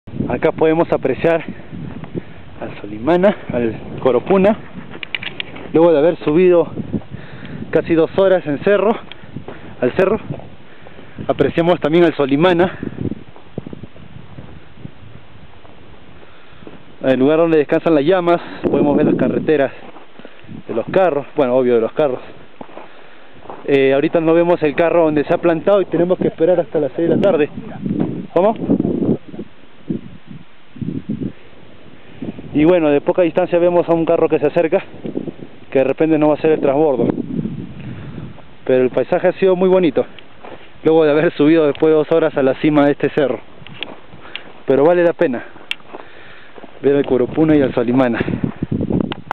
coropuna y solimana
vista desde la cima de un cerro , al coropuna y al solimana